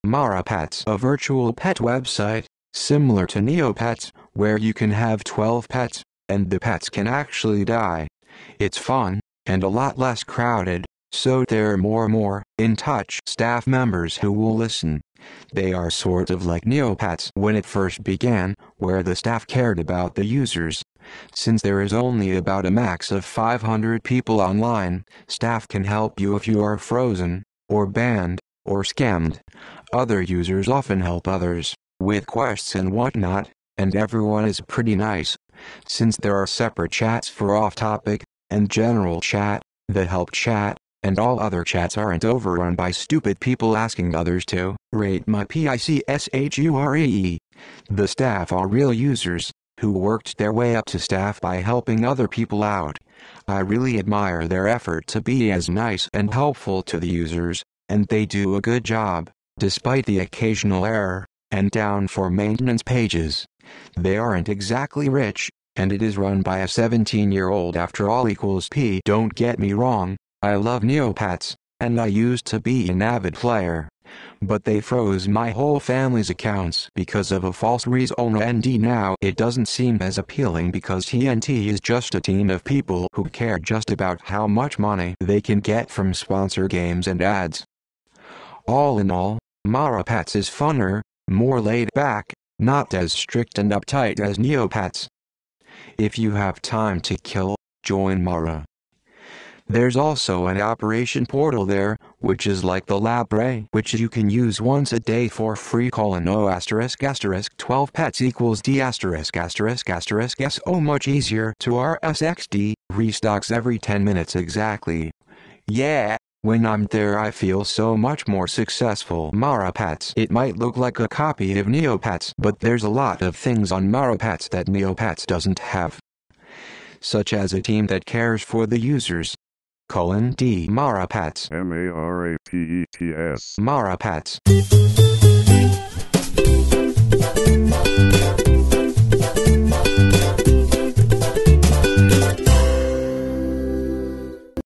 marapets meaning and pronunciation
A virtual pet website, similar to Neopets, where you can have 12 pets, and the pets can /actually/ die. It's fun, and a lot less crowded, so there more more "in touch" staff members who WILL listen. They are sort of like Neopets when it first began, where the staff cared about the users. Since there is only about a max of 500 people online, Staff can help you if you are frozen, or banned, or scammed. Other users often help others, with quests and whatnot, and everyone is pretty nice ^-^. Since there are separate chats for off topic, and general chat, the help chat, and all other chats aren't overrun by stupid people asking others to "RATE MY PICSHUREE". The staff are real users, who worked their way up to staff by helping other people out. I really admire their effort to be as nice and helpful to the users, and they do a good job, despite the occasional error... and down for maintenence pages. They aren't exactly rich, and it is run by a 17 year old after all =P. Don't get me wrong, I love neopets, and I used to be an avid player. But they froze my whole family's accounts because of a false reasona nd now it doesn't seem as appealing because TNT is just a team of people who care just about how much money they can get from sponsor games and ads.

All in all, Marapets is funner, more laid back, not as strict and uptight as Neopets :]. If you have time to kill, join mara ^^

* There's also an operation portal there, which is like the lab ray which you can use once a day for free :o
** 12 pets =D
*** SO much easier to RS XD (restocks every 10 minutes exactly)

Yeah, when I'm there I feel so much more successful.

marapets definition by Urban Dictionary